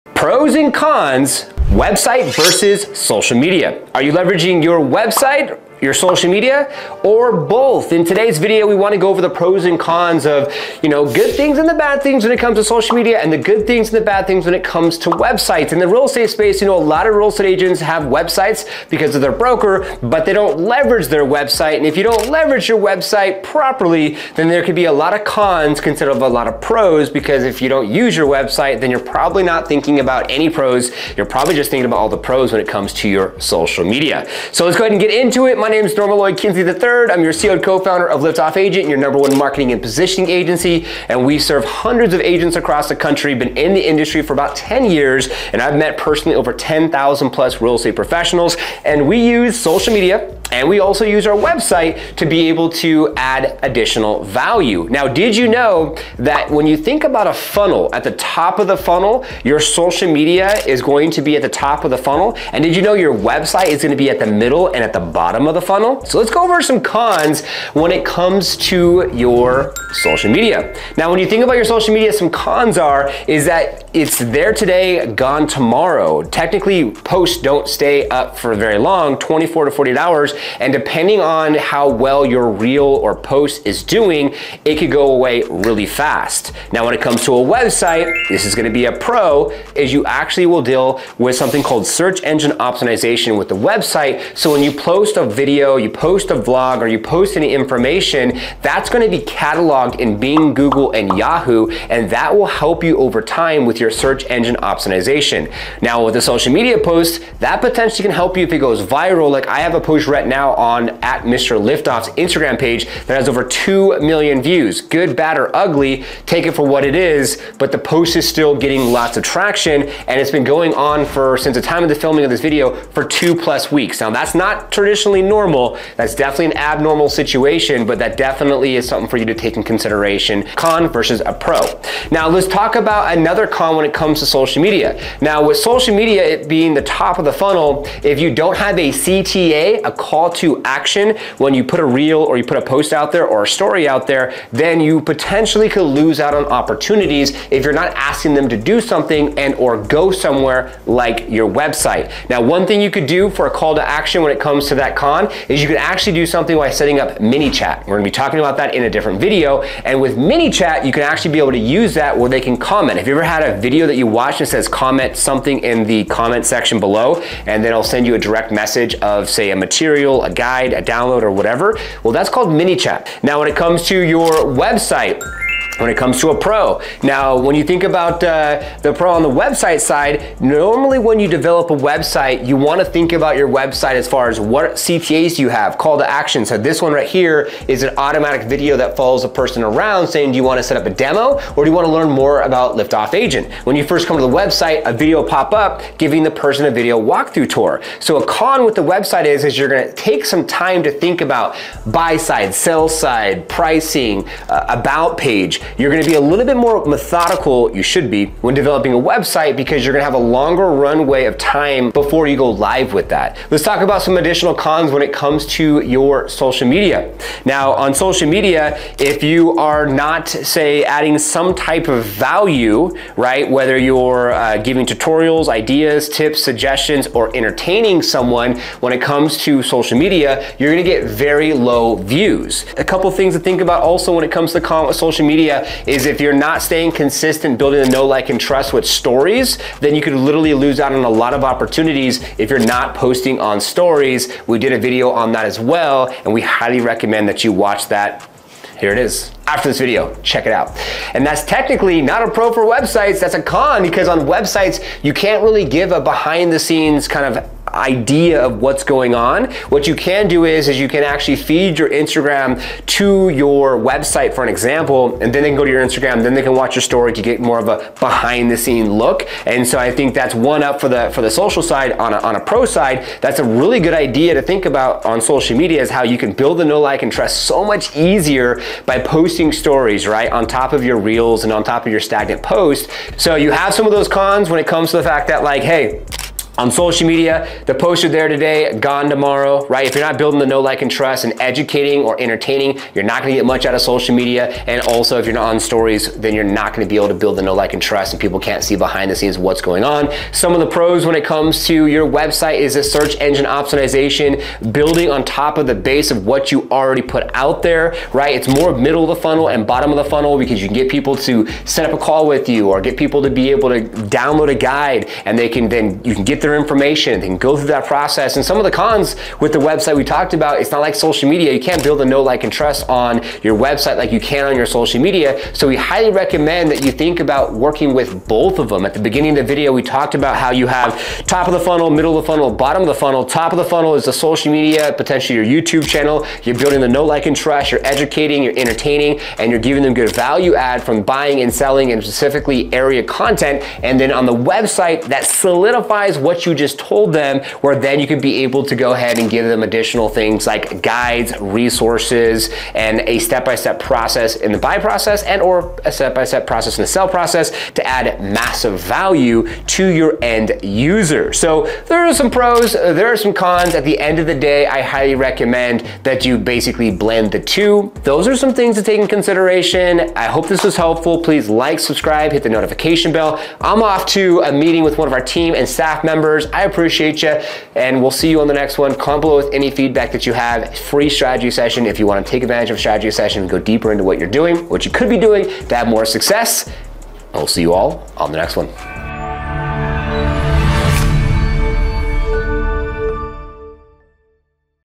The Pros And Cons Of Having A Website VS Social Media For Real Estate Agents
Are you getting the most out of your website and social media? As a real estate agent, knowing the pros and cons of each platform can make a big difference in your business. In this video, I break down the benefits and drawbacks of using websites and social media, so you can make the best choice for your needs.

Timestamps:
00:00 - Introduction
00:26 - Importance of Leveraging Both Platforms
01:18 - Cons of Social Media
02:14 - Call to Action on Social Media
03:07 - Pros of Websites
04:00 - Engagement Challenges on Social Media
04:27 - Advantages of Social Media Engagement
05:15 - Integrating Websites and Social Media

We're here to keep adding value to your real estate journey!

💥FREE RESOURCES TO START YOUR SUCCESS IN REAL ESTATE 💥